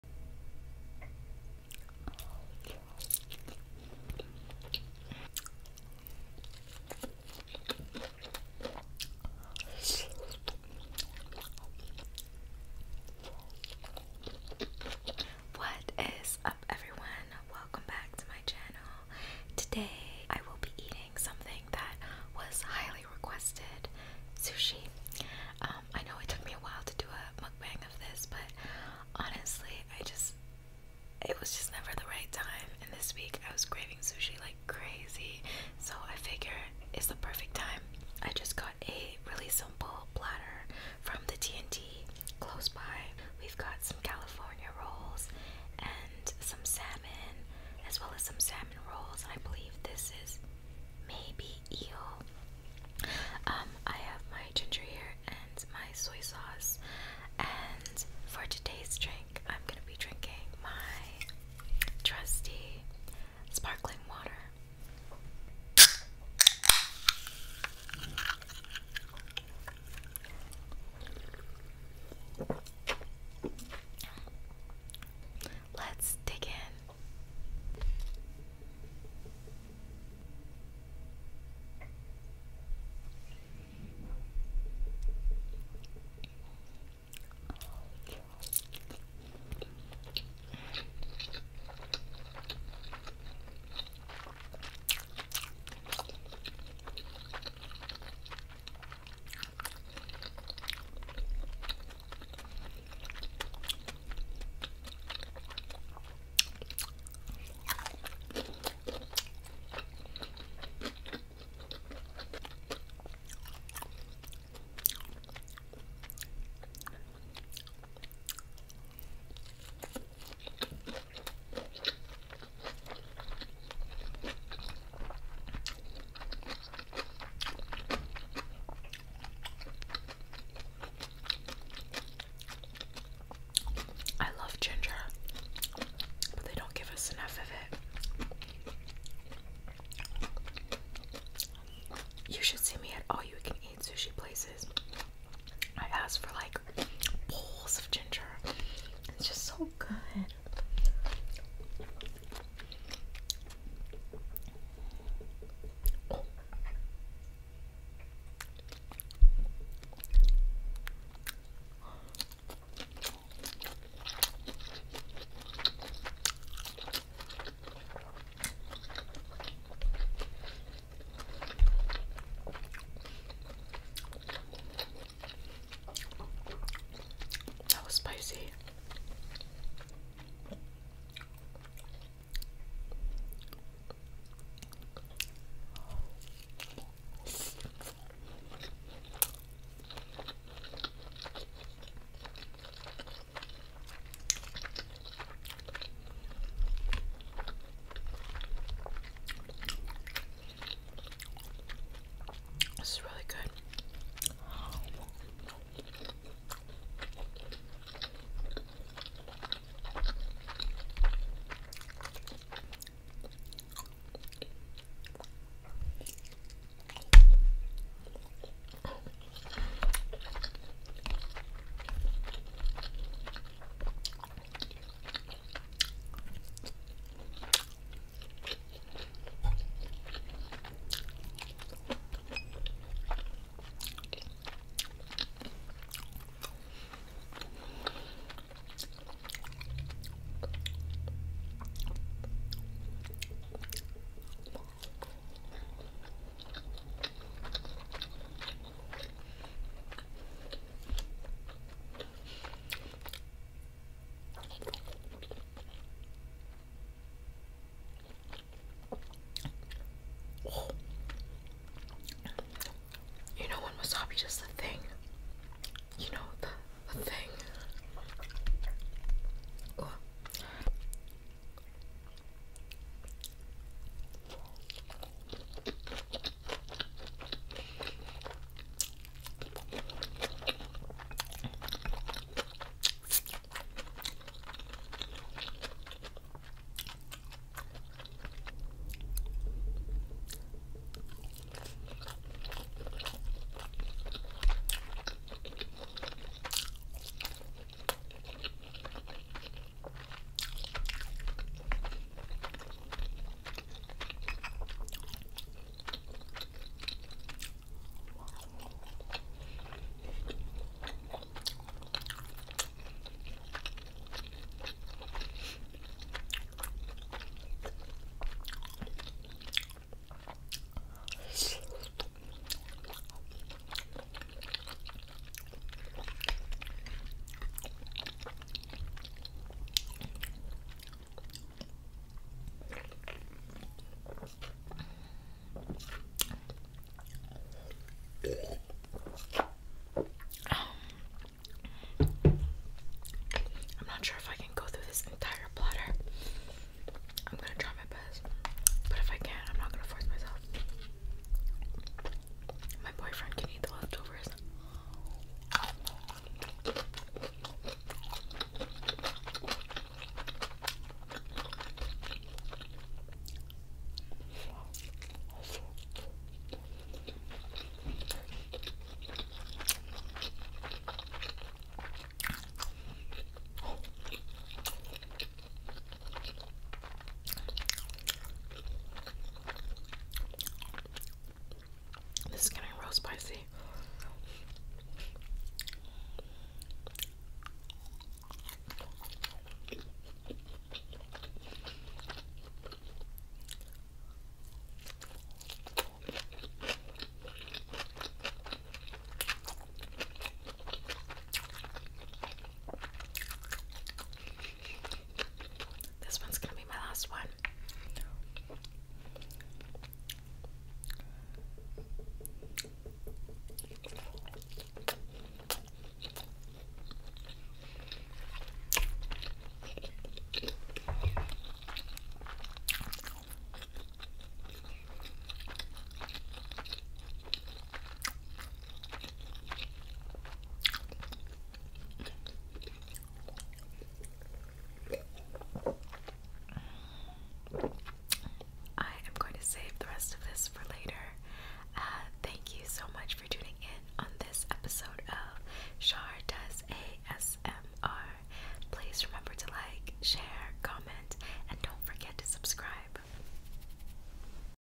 ASMR SUSHI ~ asmr whispering
Hey y’all! Today I will be eating SUSHI! I hope you enjoy.